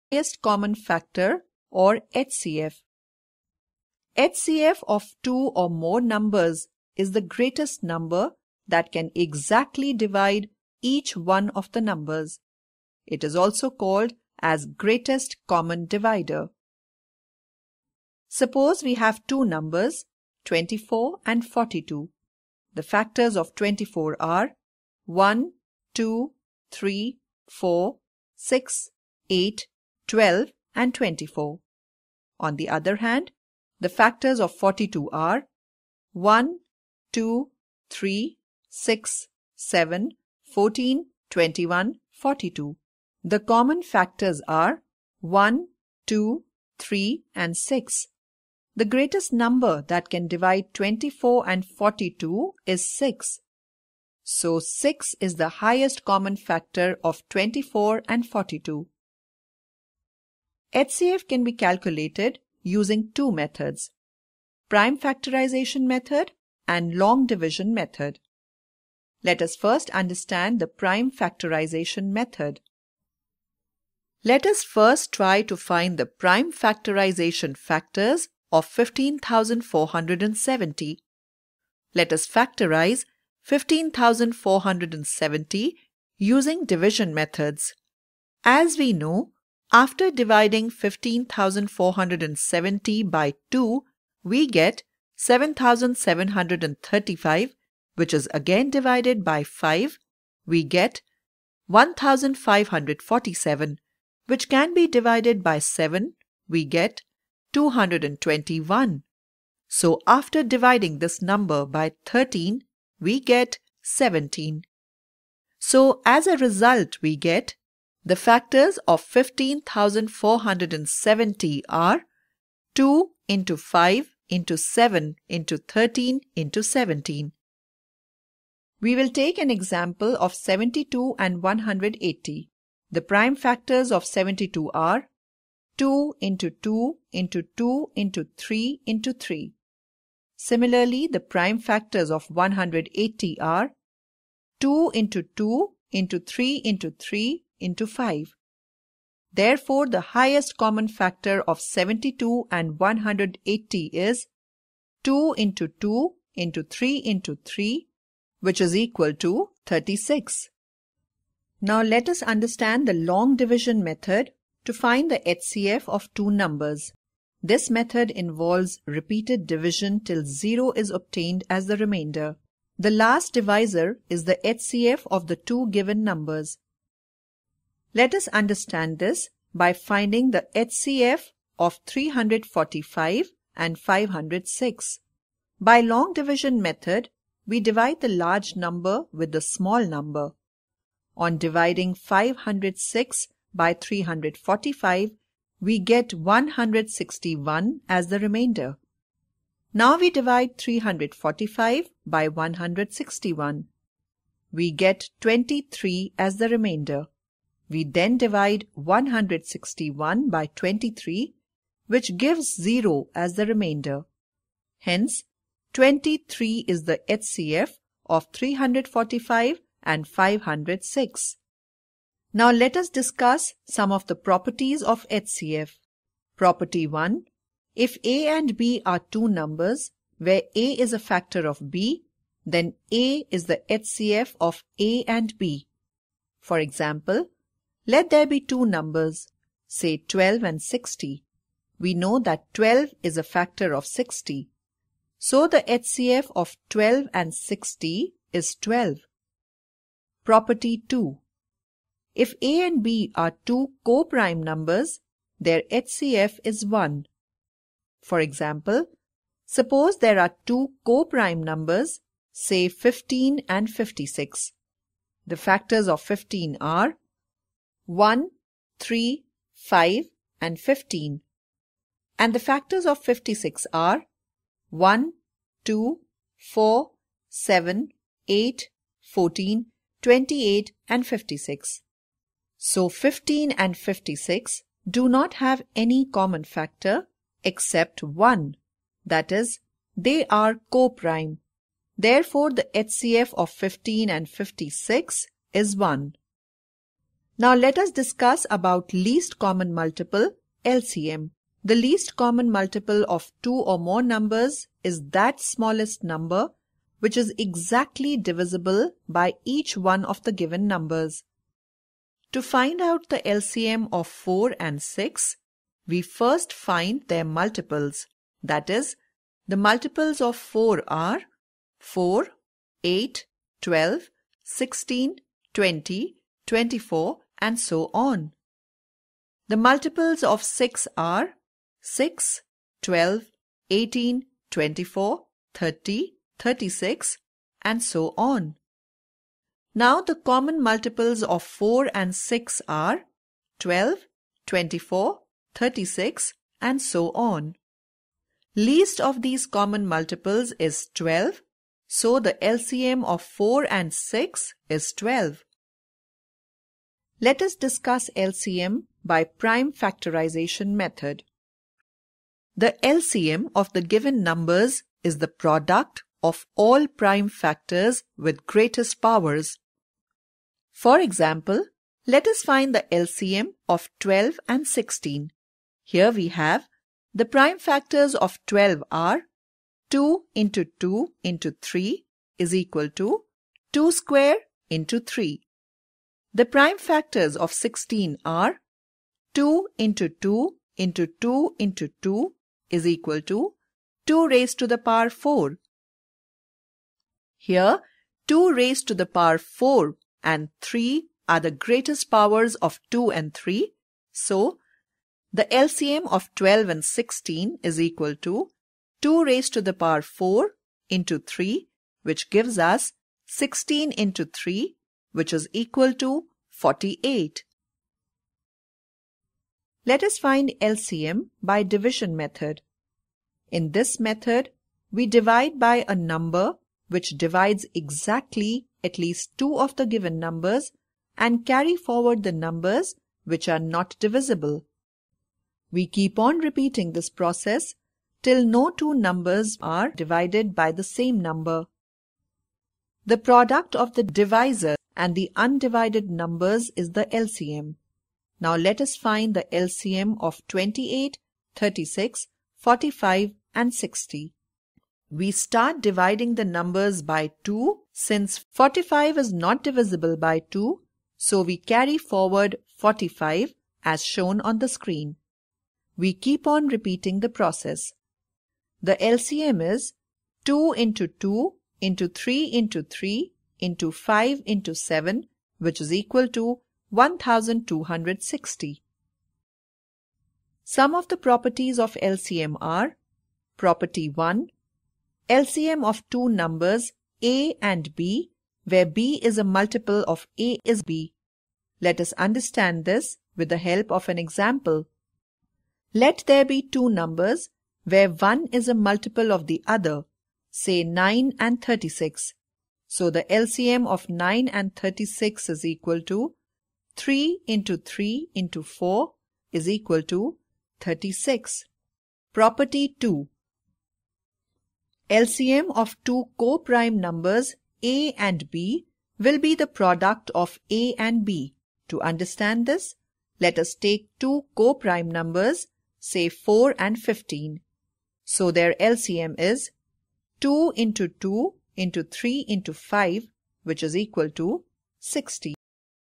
HCF and LMC- Prime Factorization method, Long Division Method,
Finding the HCF of numbers using the prime factorization method and the long division method. Properties of HCF and LCM of numbers